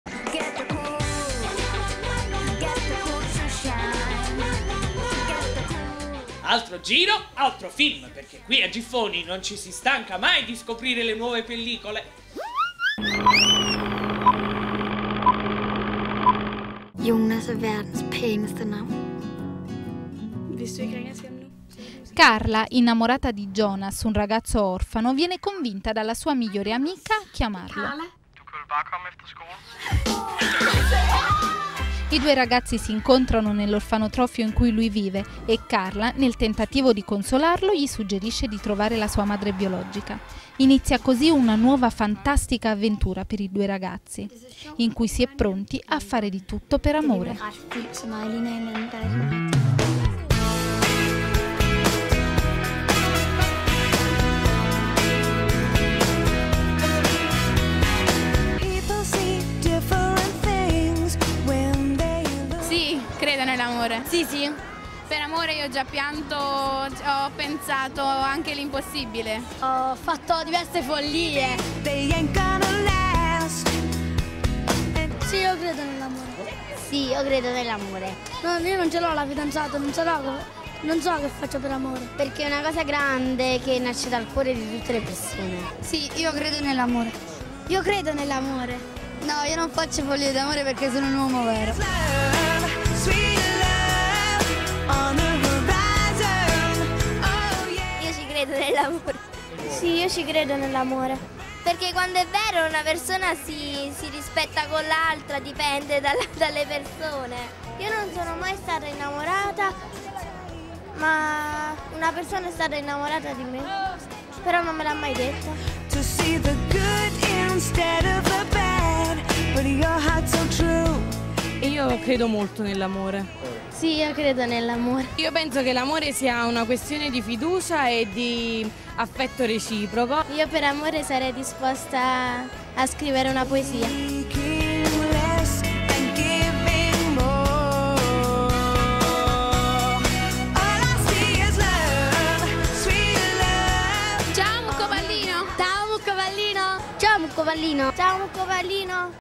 Al Giffoni Experience... il film: "Karla and Jonas"
"Karla and Jonas" è la storia d'amore di 2 ragazzi che si incontrano in un orfanotrofio, la cui trama, dunque, diventa occasione, per MUKKO PALLINO, per analizzare la fiducia che i piccoli giurati nutrono nell'amore!